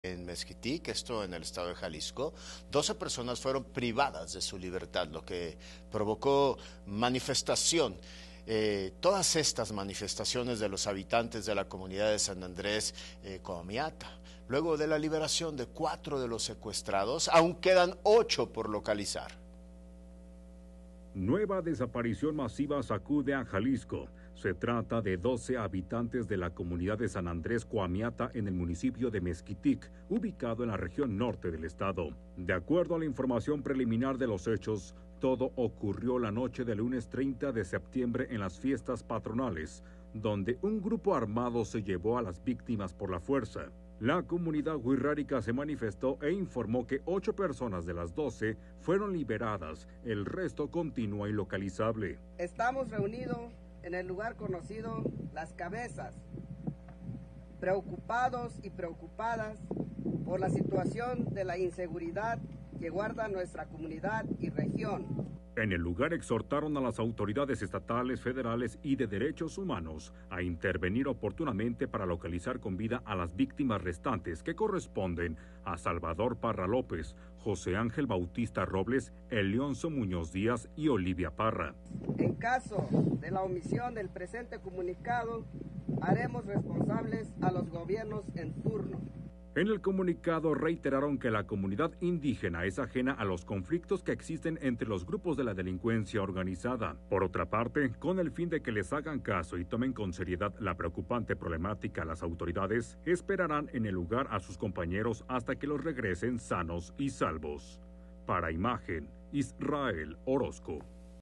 12 personas fueron secuestradas en Mezquitic, Jalisco | Noticias GDL con Rey Suárez
Un grupo armado secuestró a 12 habitantes de la comunidad de San Andrés Cohamiata, en el municipio de Mezquitic, Jalisco.